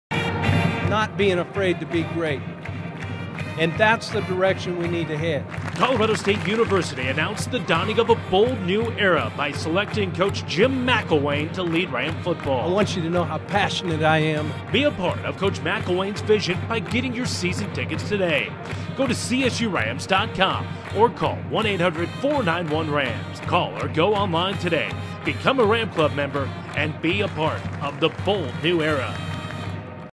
It's a Bold New Era for Ram Football
A Bold New Era for Ram football has begun at Colorado State University, and the CSU Department of Athletics is giving fans an opportunity to guarantee great seat locations when new Head Coach Jim McElwain and the 2012 Rams take the field at Sonny Lubick Field at Hughes Stadium.

The athletics ticket office will immediately begin taking deposits on new 2012 season-ticket orders. For just $50 per seat, fans can reserve great seat locations when full season-ticket packages become available in the spring.